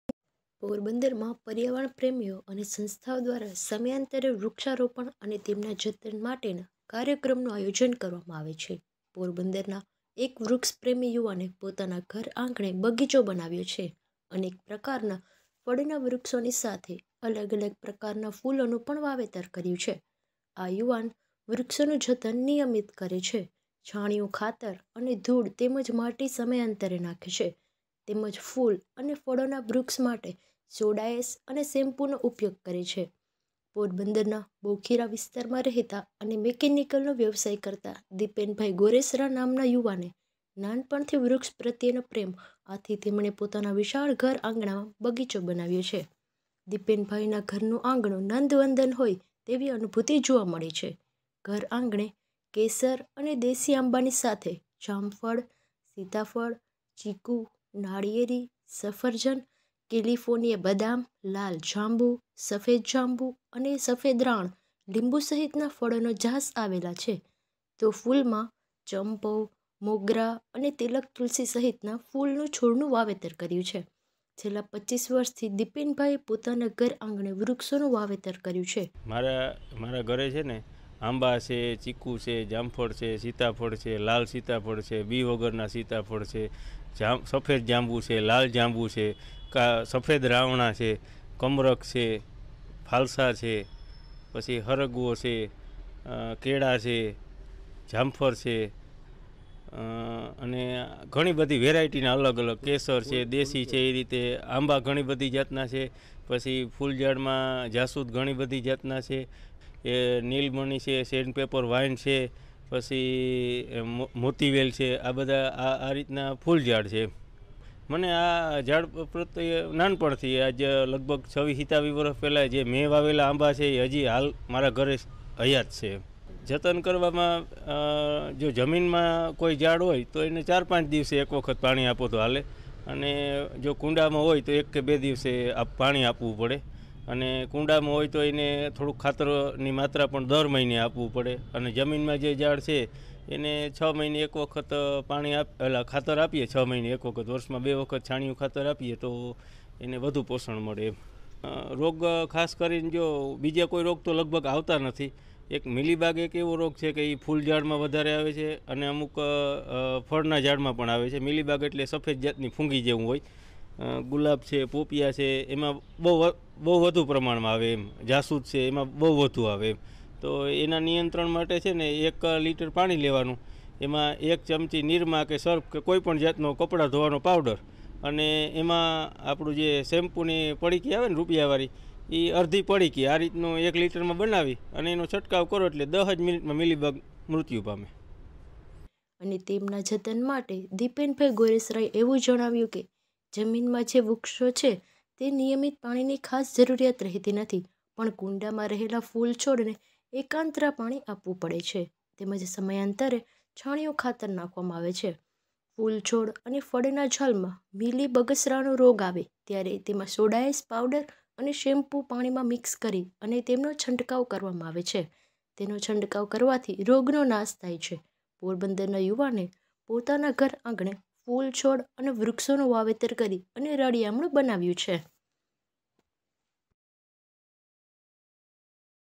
Porbandar News | પોરબંદરનાં યુવાને ફળિયાને બનાવ્યું નંદનવન, આવી રીતે કરે છે જતન
The youth of Porbandar has made a paradise in Paliya, this is how Jatan works

પોરબંદરનાં યુવાને ફળિયાને બનાવ્યું નંદનવન, આવી રીતે કરે છે જતન